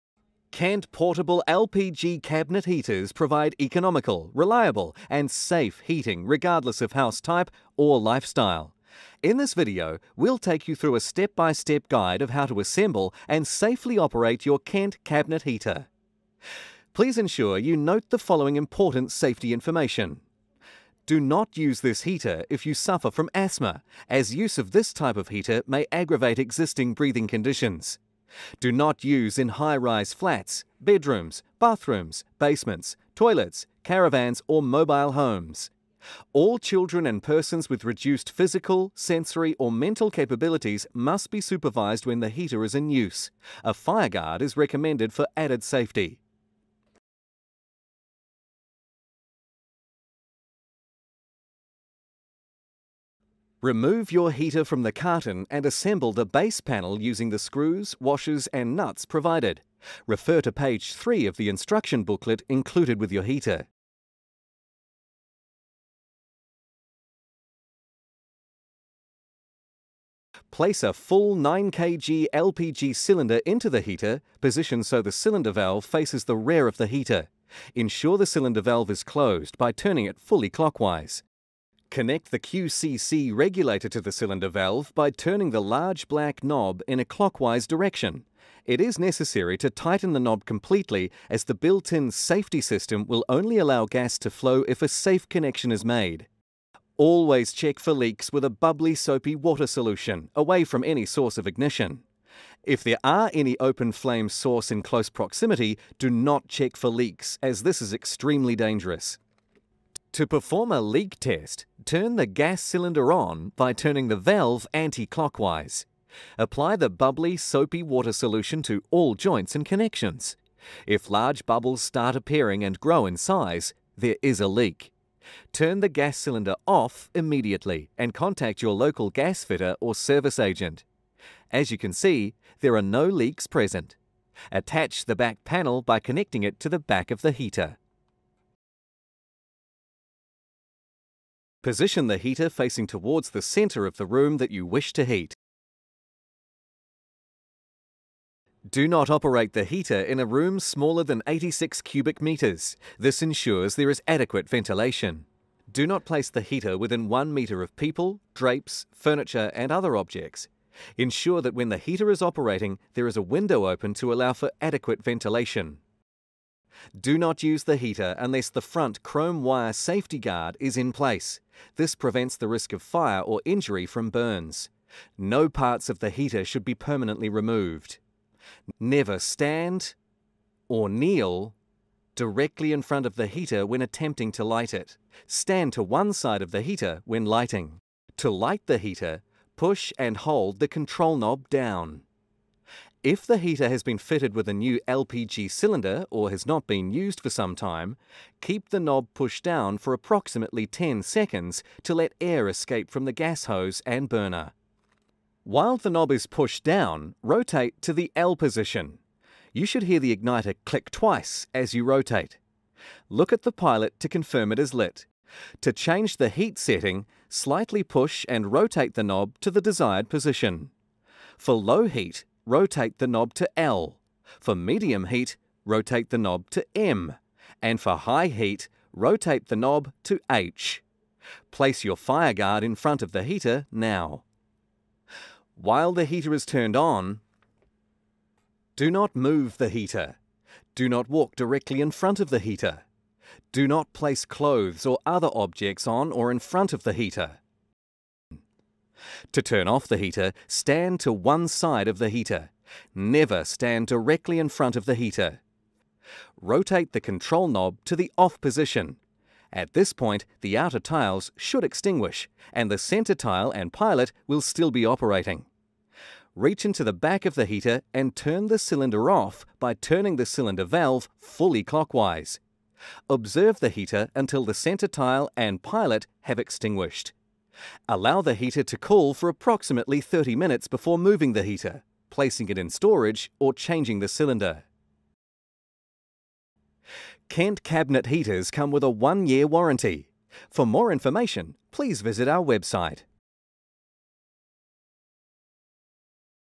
Kent Eclipse LPG Cabinet Heater
Learn how to assemble and safely operate our Kent Eclipse LPG Cabinet Heater.